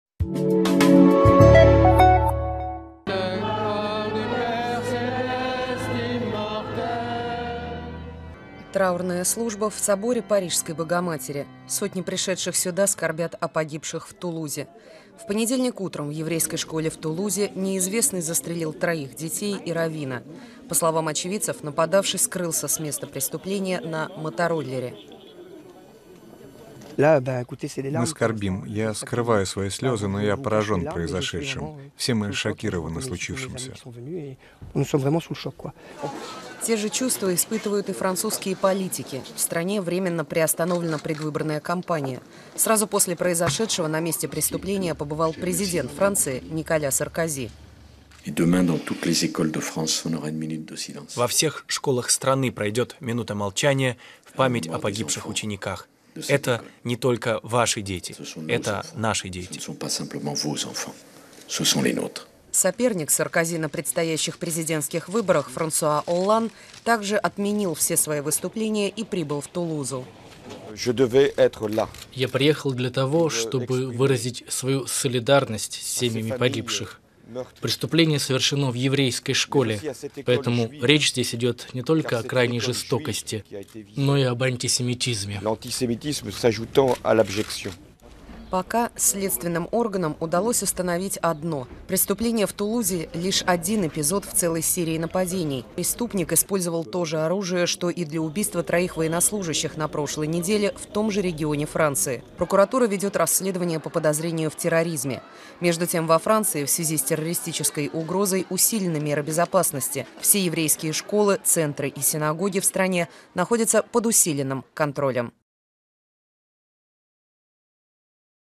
Франция шокирована трагедией в Тулузе
После убийства четырех человек во французской Тулузе на юге страны объявлен высший уровень террористической угрозы. Трагедия стала уже третьим подобным нападением за последние несколько дней. Ведется следствие.